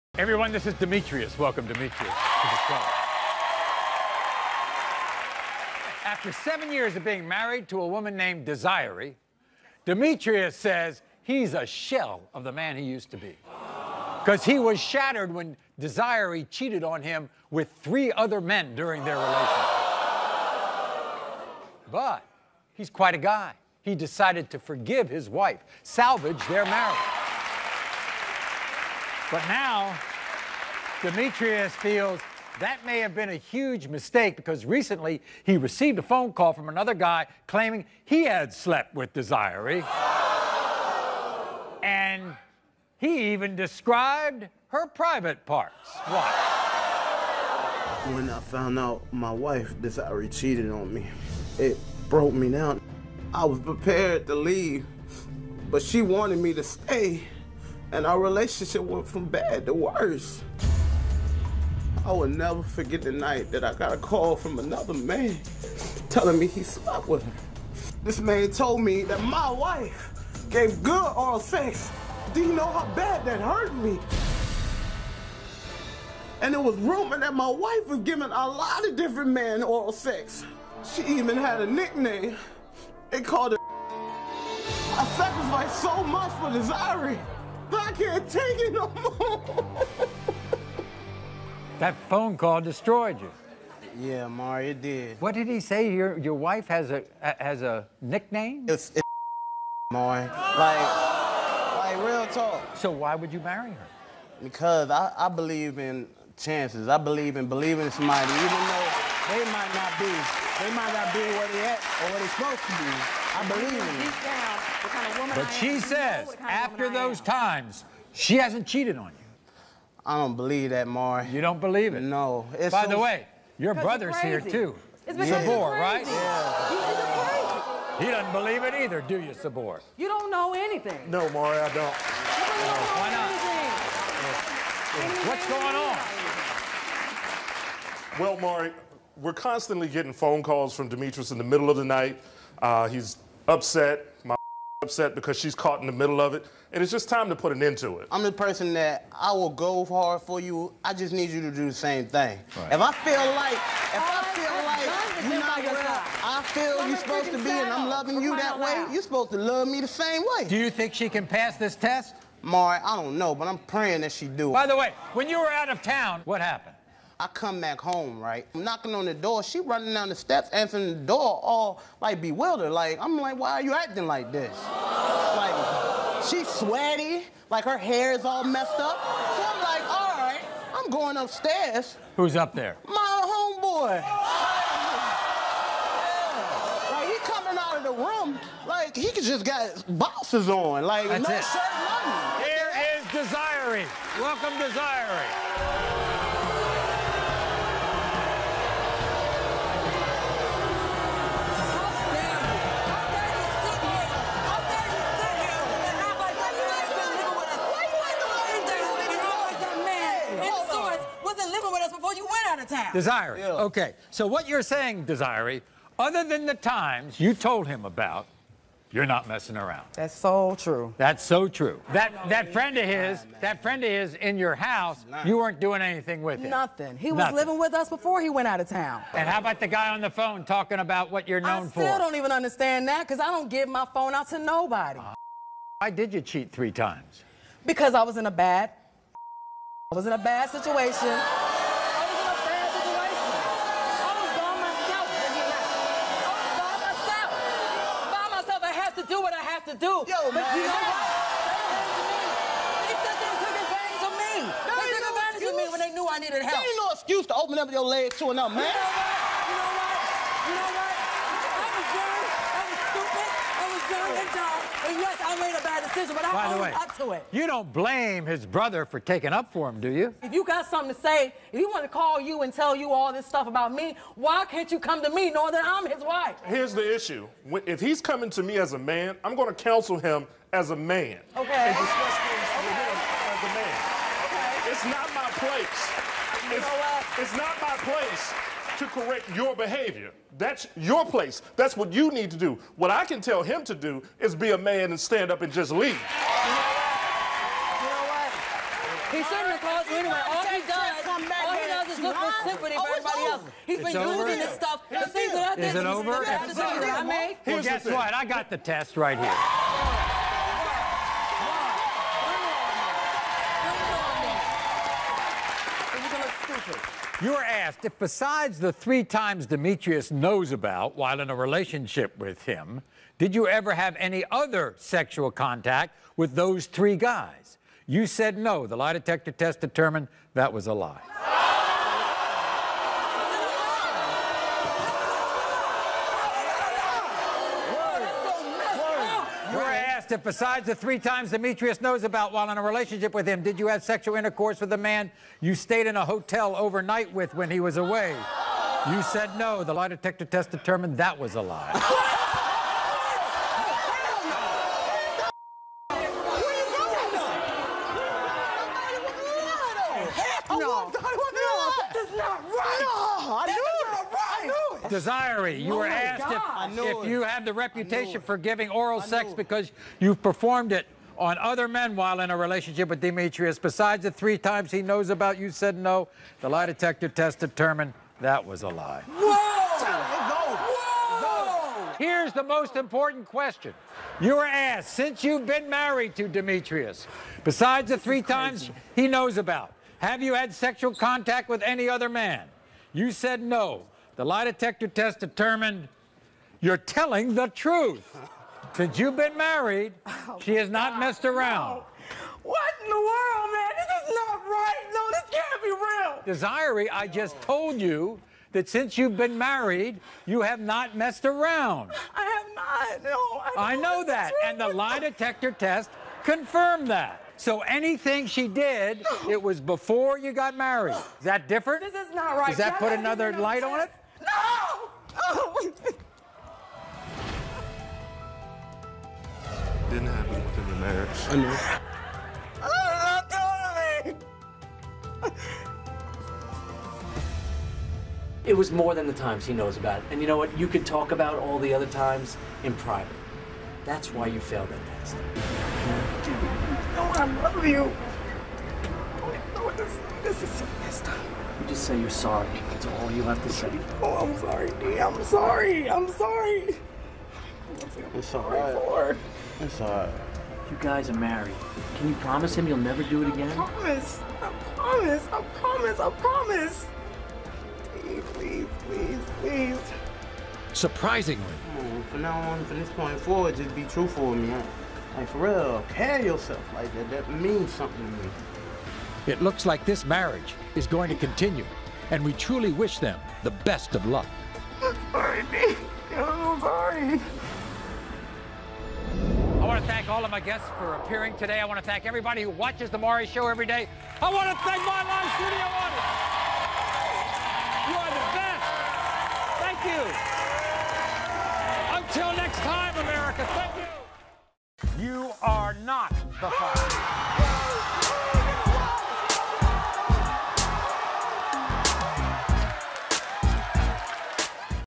Cheating Before Marriage Doesn’t Count!
Demetrius believes that his wife Desiree has been cheating on him! But Desiree says it only happened once…before they were married!